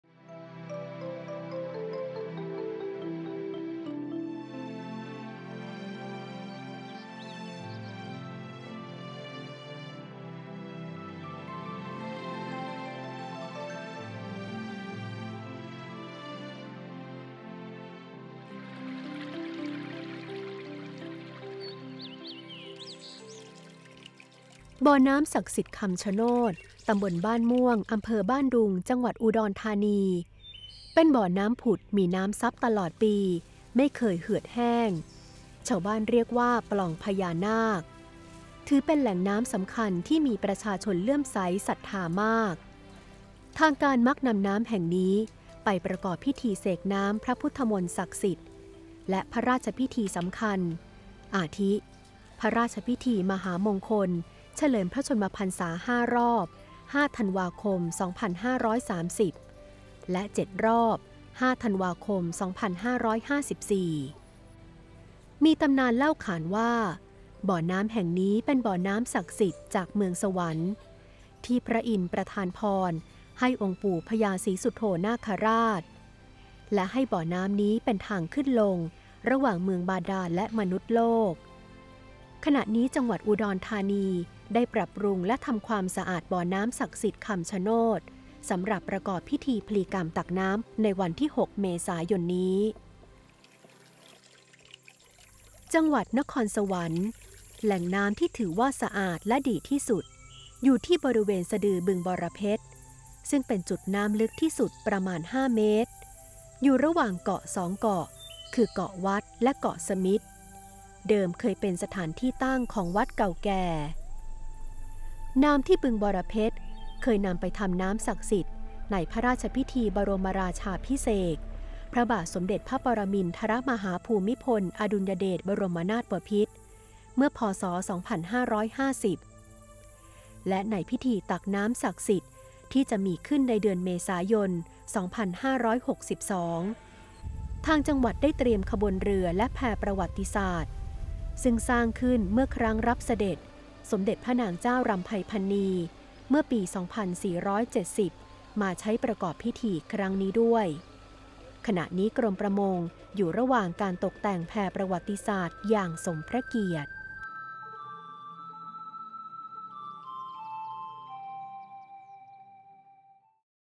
สารคดี แหล่งน้ำศักดิ์สิทธิ์ ตอนที่ 17 ในจังหวัดอุดรธานีและจังหวัดนครสวรรค์
ดูรายละเอียดเพิ่มเติม